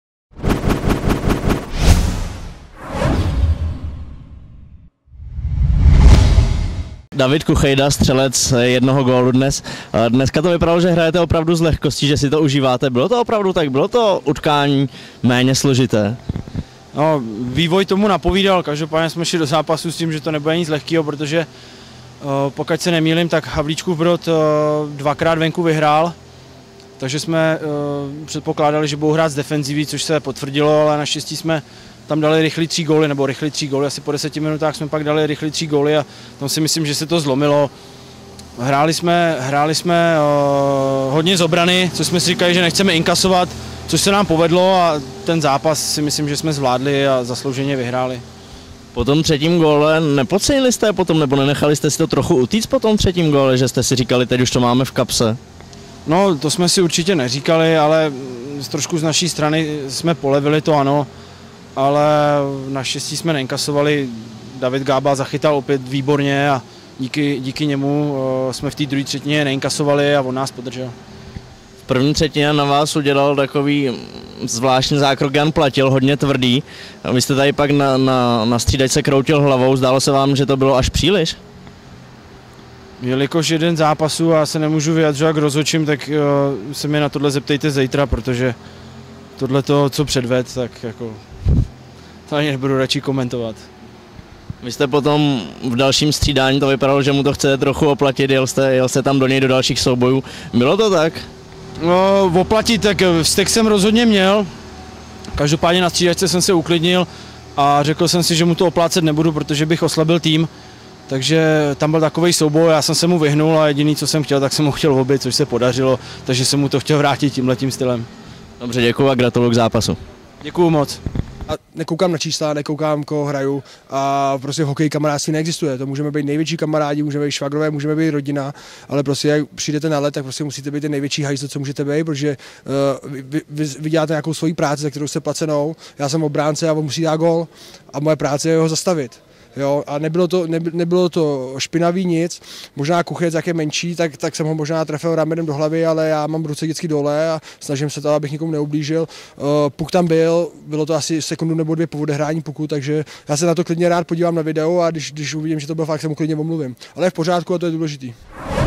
Rozhovor: ČEZ Motor České Budějovice - HC Rebel Havlíčkův Brod
Pozápasový rozhovor s Davidem Kuchejdou a Janem Platilem